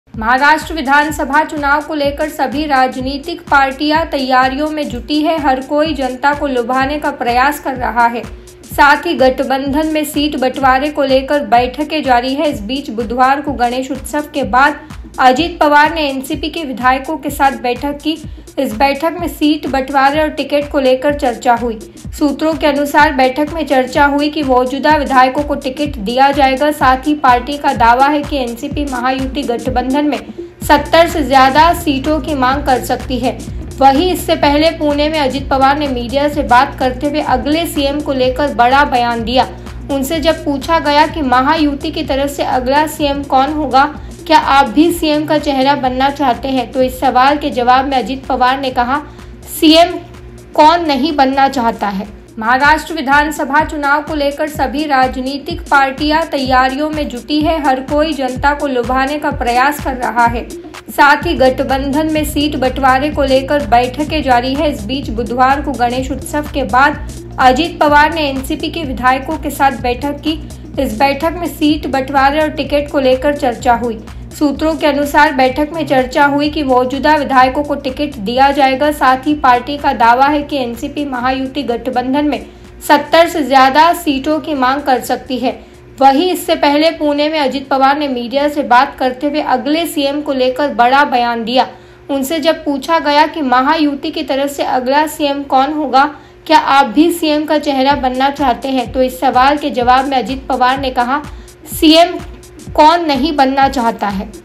Ajit Pawar सीएम बनने के सवाल पर बोले सीएम कौन नहीं बनना चाहता है
महाराष्ट्र विधानसभा चुनाव को लेकर सभी राजनीतिक पार्टियां तैयारियों में जुटी हैं. हर कोई जनता को लुभाने का प्रयास कर रहा है. साथ ही गठबंधन में सीट बंटवारे को लेकर बैठकें जारी हैं. इस बीच बुधवार को गणेशोत्सव के बाद अजित पवार ने एनसीपी के विधायकों के साथ बैठक की. इस बैठक में सीट बंटवारे और टिकट को लेकर चर्चा हुई. सूत्रों के अनुसार बैठक में चर्चा हुई कि मौजूदा विधायकों को टिकट दिया जाएगा. साथ ही पार्टी का दावा है कि एनसीपी महायुति गठबंधन में 70 से ज्यादा सीटों की मांग कर सकती है.  वहीं इससे पहले पुणे में अजित पवार ने मीडिया से बात करते हुए अगले सीएम को लेकर बड़ा बयान दिया. उनसे जब पूछा गया कि महायुति की तरफ से अगला सीएम कौन होगा? क्या आप भी सीएम का चेहरा बनना चाहते हैं? तो इस सवाल के जवाब में अजित पवार ने कहा, "सीएम कौन नहीं बनना चाहता है."